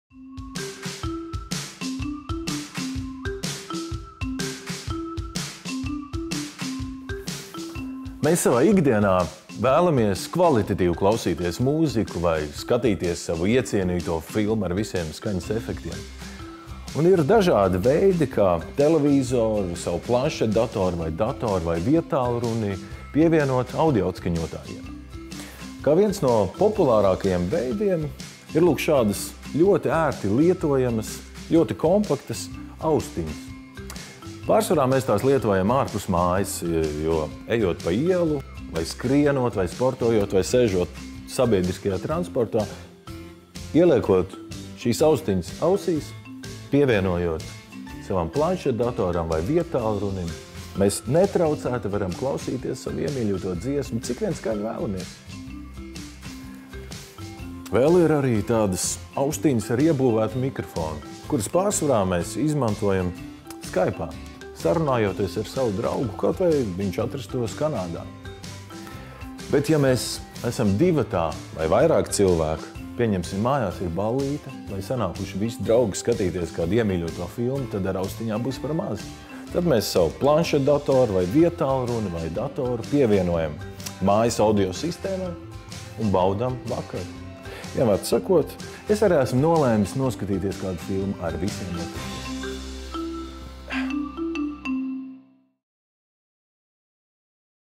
AUDIO ATSKAŅOTĀJI
Mūsdienās vēlamies baudīt kvalitatīvi skanošu mūziku, skatīties mīļāko filmu ar skaņas efektiem, tad tādas iespējas ir vairākas kā savienot savu televizoru, viedtālruni vai planšetdatoru ar kādu no audio atskaņotājiem. Tie mēdz būt savienojumi caur USB, bluetooth, HDMI kabeli, Wi-Fi.